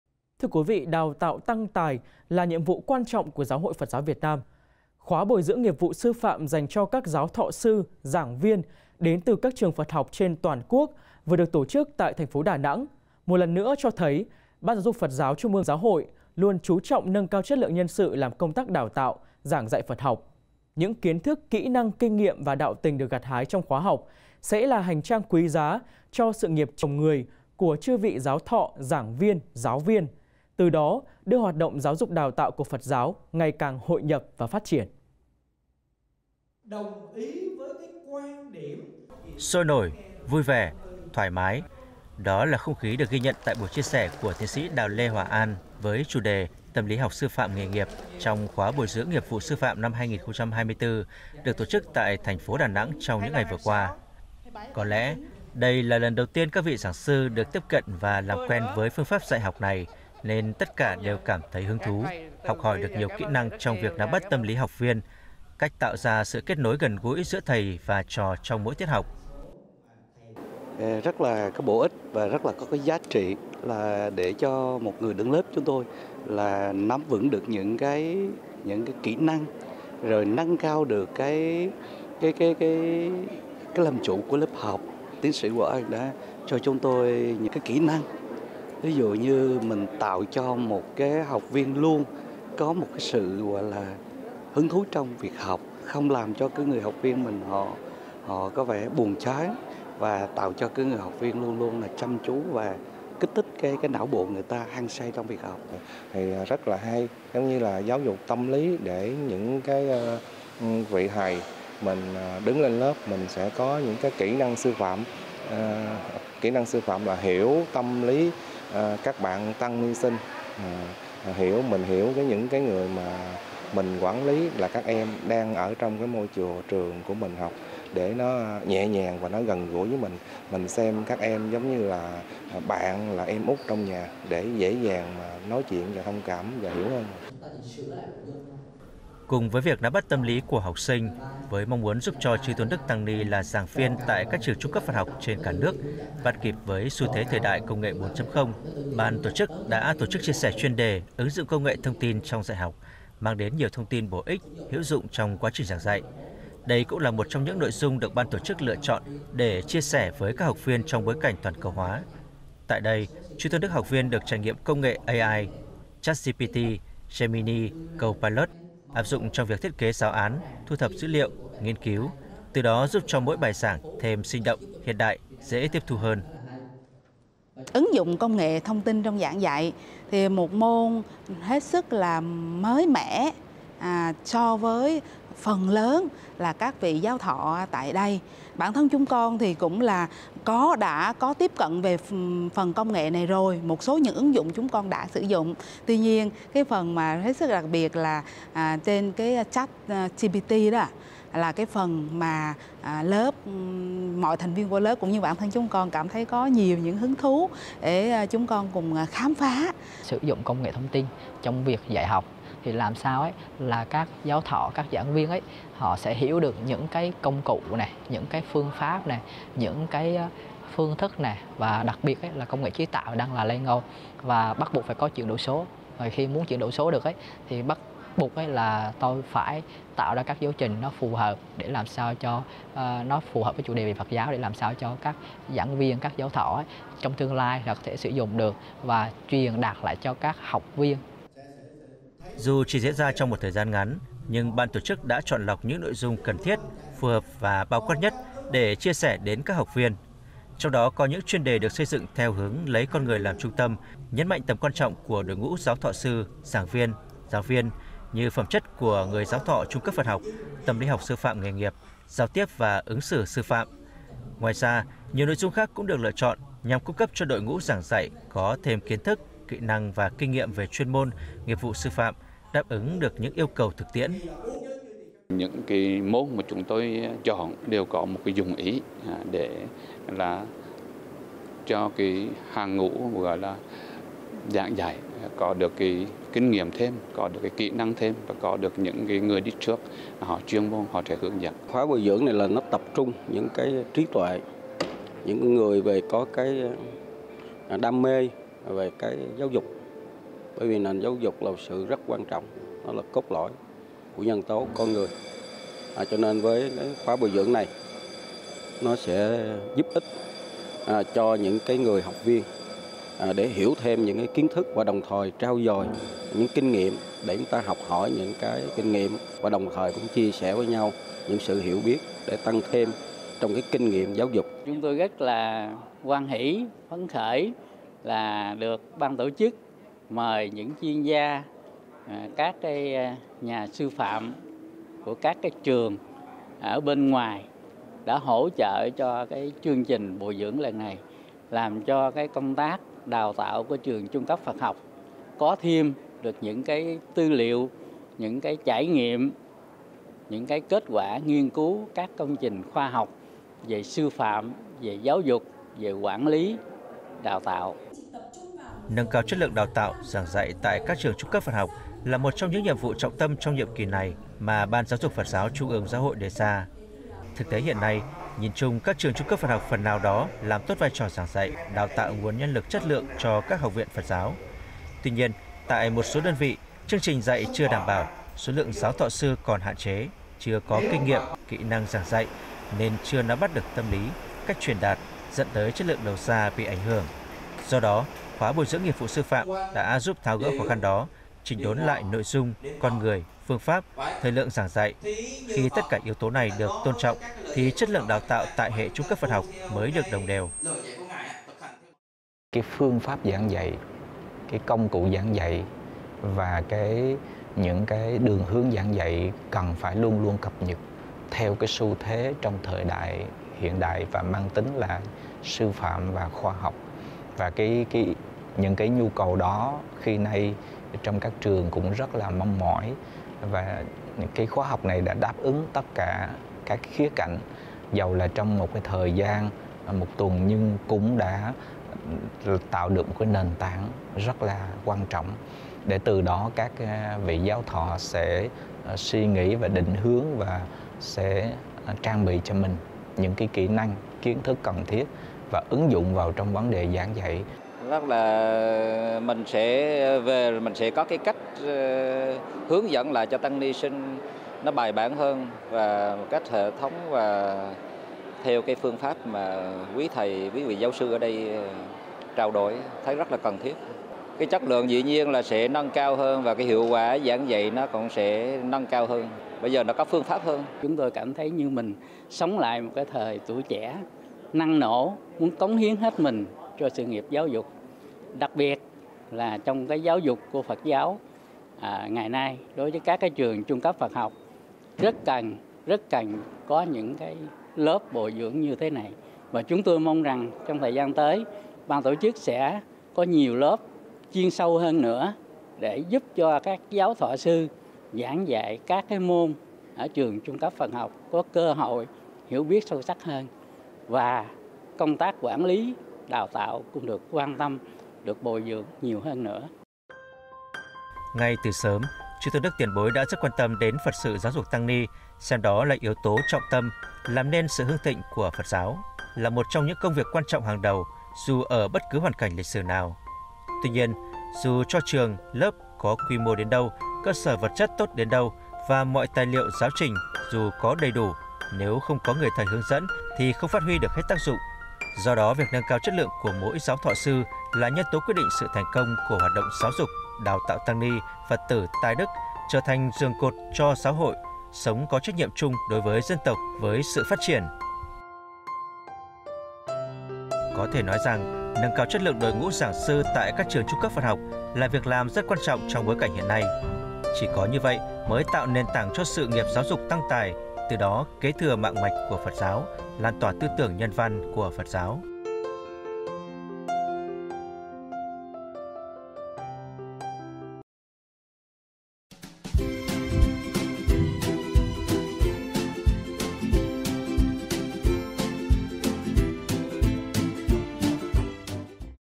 Tiêu Điểm: Nâng cao chất lượng giáo thọ sư | Bchannel
Đào tạo Tăng tài là nhiệm vụ quan trọng của GHPGVN. Khóa bồi dưỡng nghiệp vụ sư phạm dành cho các giáo thọ sư, giảng viên đến từ các trường Phật học trên toàn quốc vừa được tổ chức tại TP.Đà Nẵng một lần nữa cho thấy, Ban giáo dục Phật giáo TƯGH luôn chú trọng nâng cao chất lượng nhân sự làm công tác đào tạo, giảng dạy Phật học. Những kiến thức, kỹ năng, kinh nghiệm và đạo tình được gặt hái trong khóa học sẽ là hành trang quý giá cho sự nghiệp trồng người của chư vị giáo thọ, giảng viên, giáo viên. Từ đó, đưa hoạt động Giáo dục đào tạo của Phật giáo ngày càng hội nhập và phát triển.